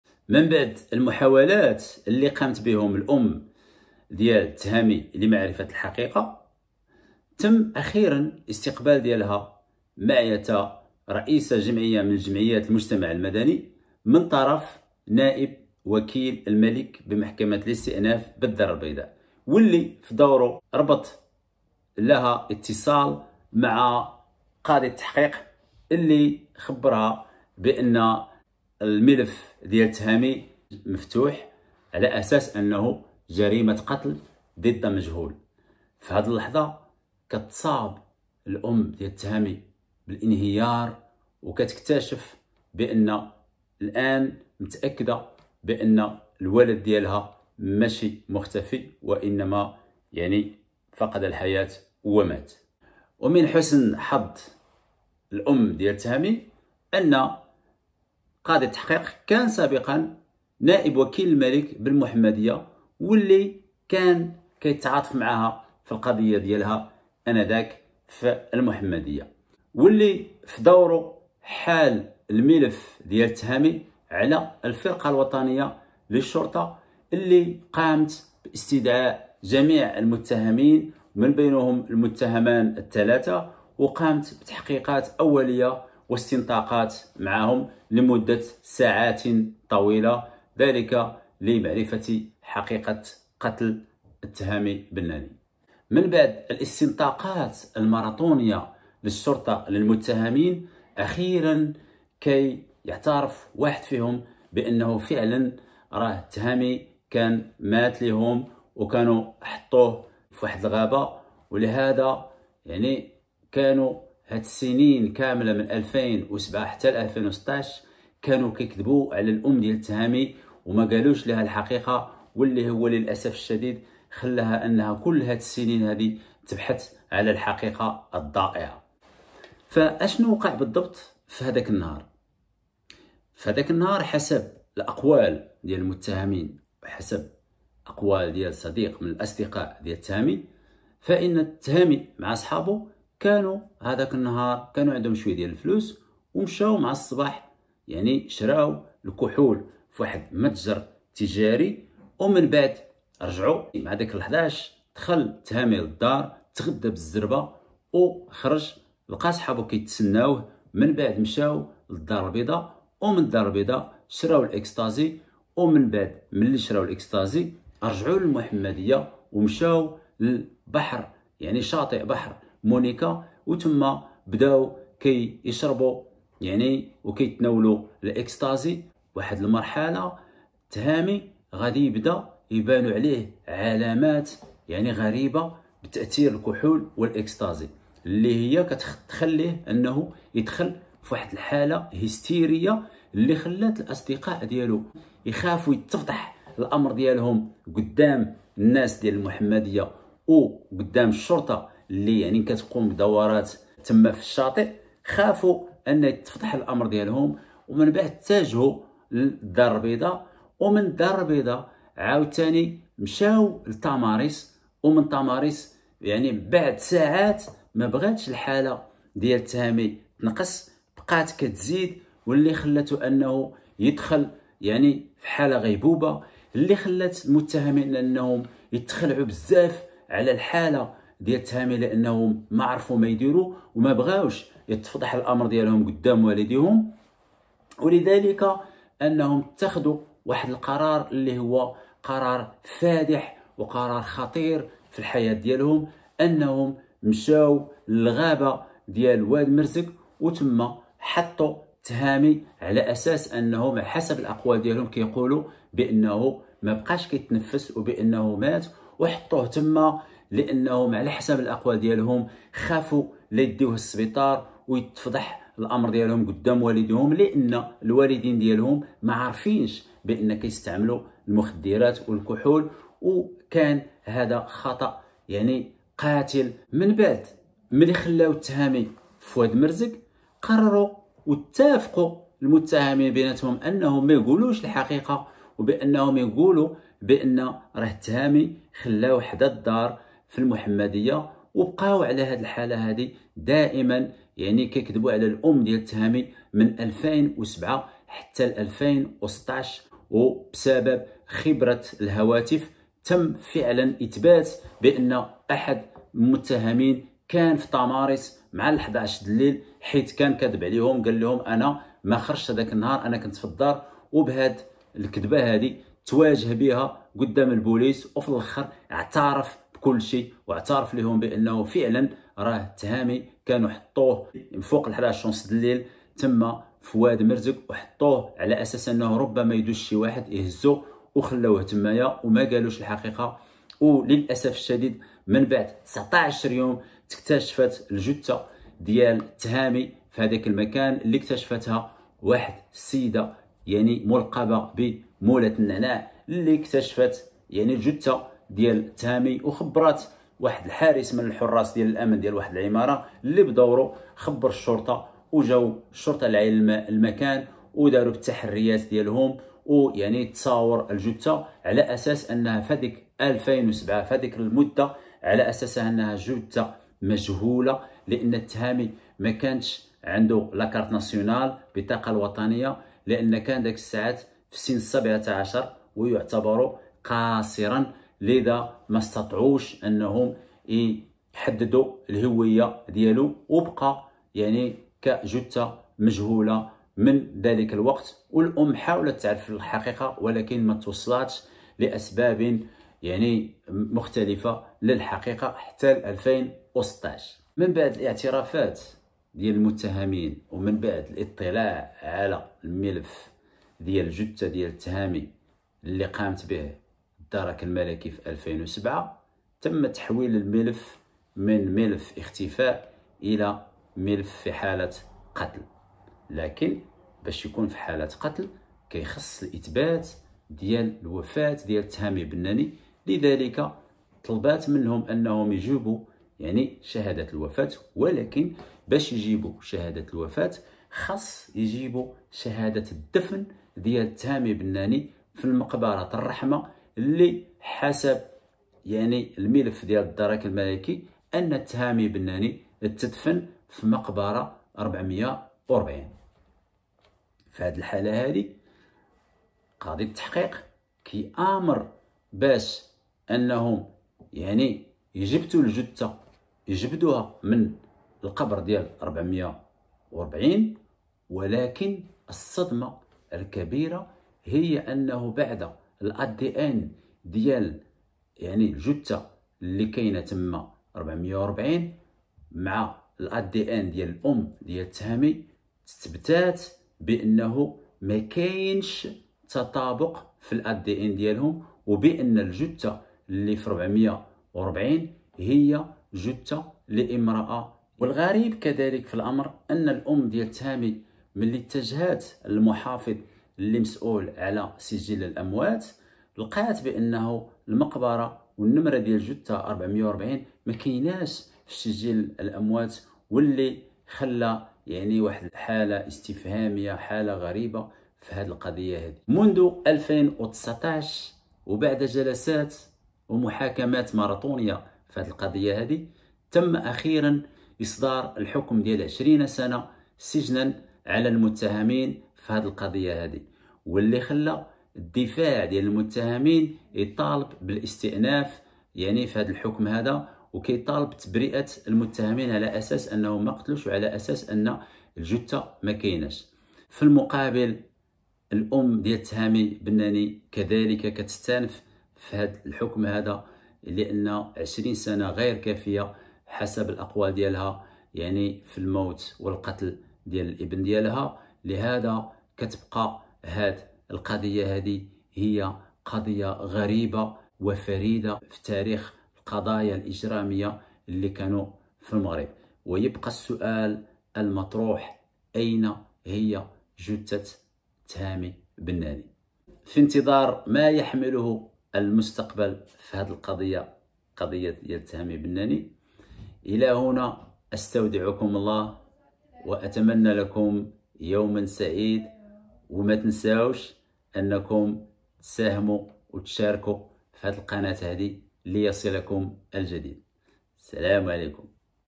التهامي بناني (الجزء الاخير): حل لغز مقتل وإخفاء الجثة منذ 2007
شاركو بآرائكم وتعالقكم حول القصة في قنات حسن حمدي ودعمونا تشجيعا منكم لنا ليصلكم المزيد من الافلام

حل لغز قتل وإخفاء جثة التهامي بناني منذ 2007 (لجزء الاول)
قصص واقعية حقيقية باسماء مختلفة احتراما لخصوصية الاشخاص